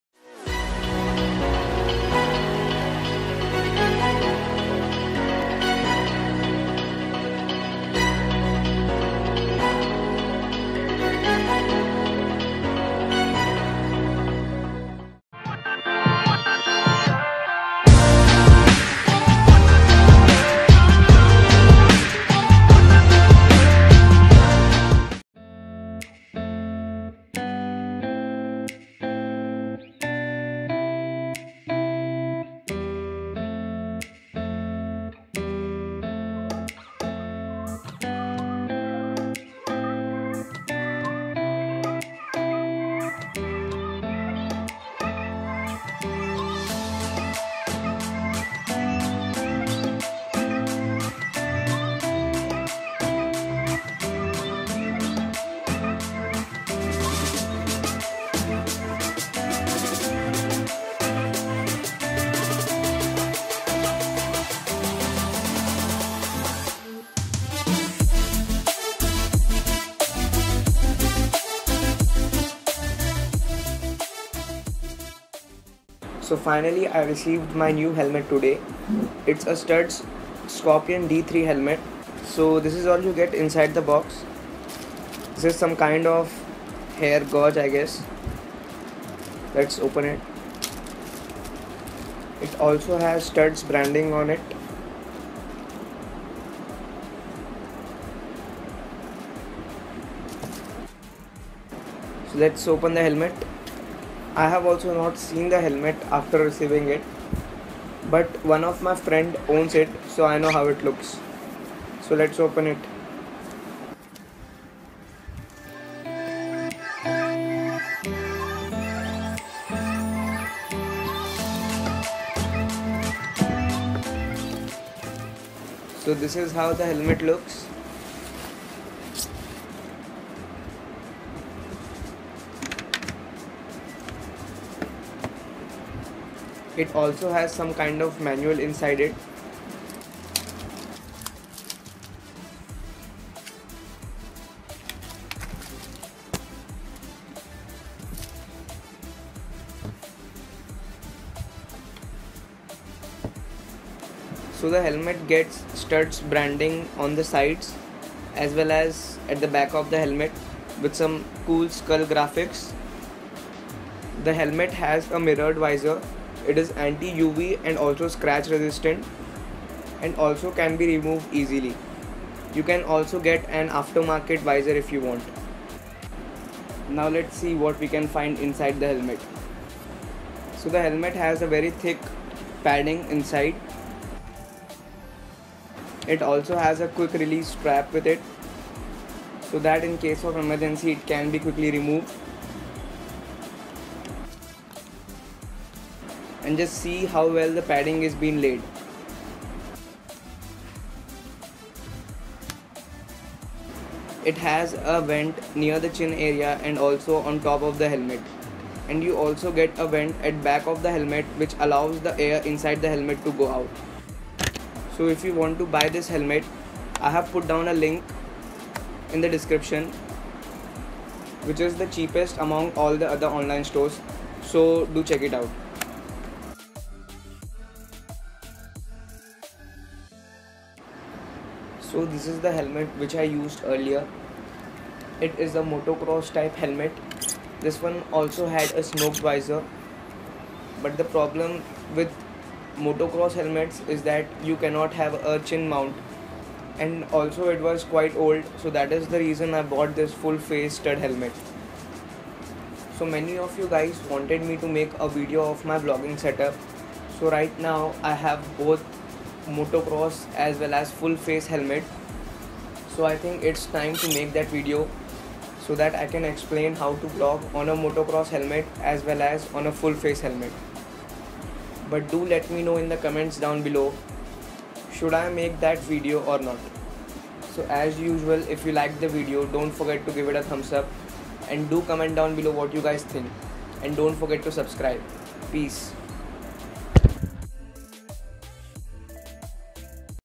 Best And Cheap Helmet In India | 2000₹ | Studds Scorpion D3 | Unboxing and Review
Yo whats up people !! So i finally got my new helmet The Studds Scorpion D3 Matte Black..So here is the unboxing and review of the helmet !!

If you like this video , throw a thumbs up.. :)